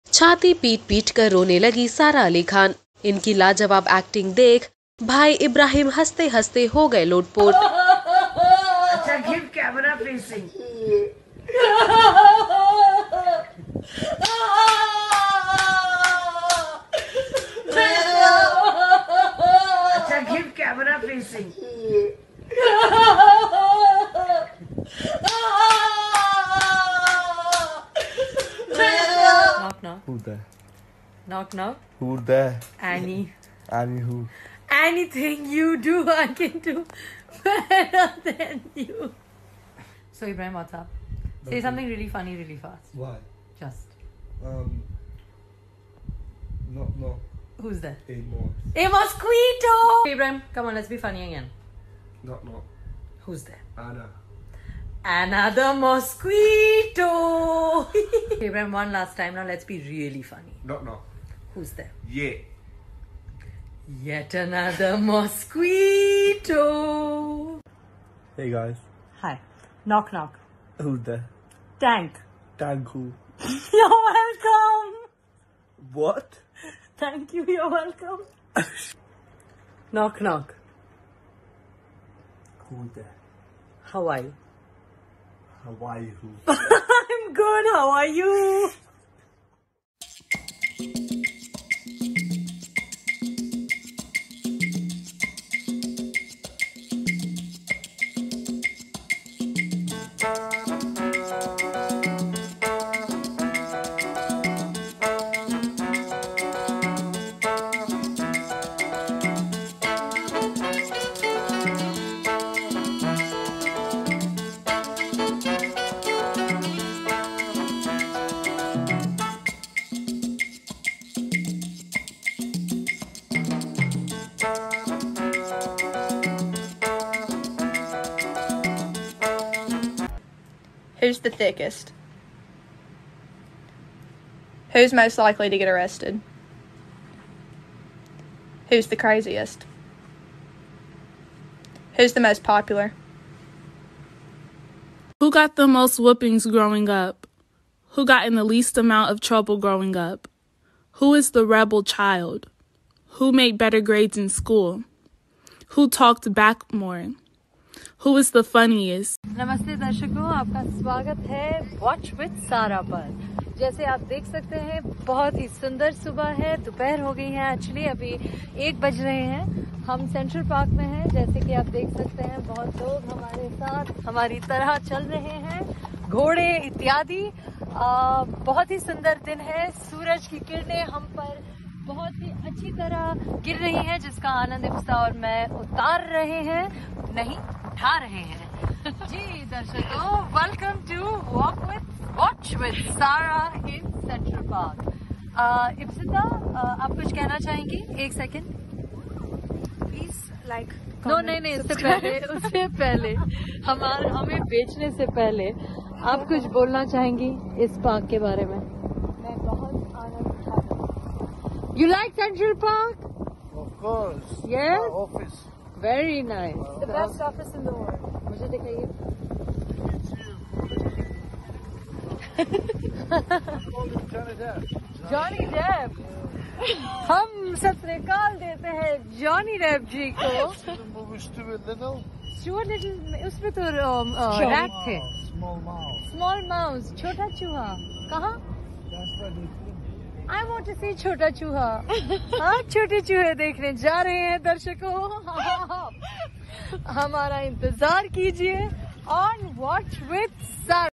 Sara Ali Khan Cries Out Loud, Ibrahim Laughs | Best EPIC Comedy Videos Of Sara With Ibrahim
Sara Ali Khan proves to be an actor since childhood as a throwback video of her has hit the internet where she is seen giving a wonderful treat to her family with a crying scene. Her brother Ibrahim uncontrollable laughter is must watch. Watch Sara Ali Khan's non stop funny videos.

Reporter & Editor: Abhishek Halder